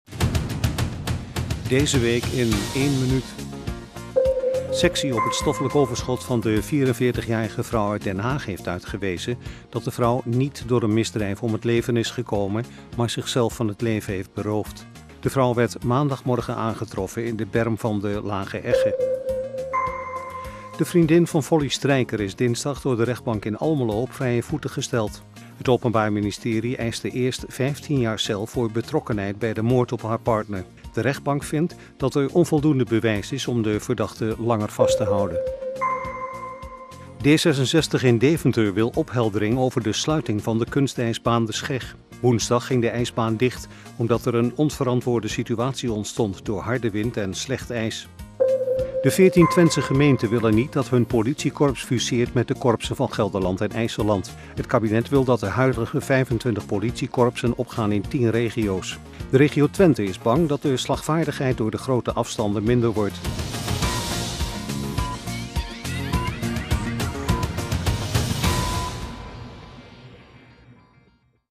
Nieuws 1 minuut weekoverzicht
- Vrouw uit Den Haag niet door misdrijf omgekomen
- Vriendin Folly Strijker op vrije voeten
- De Scheg gesloten door harde wind en slecht ijs
- Geen fusie Politie Twente met IJsselland en Gelderland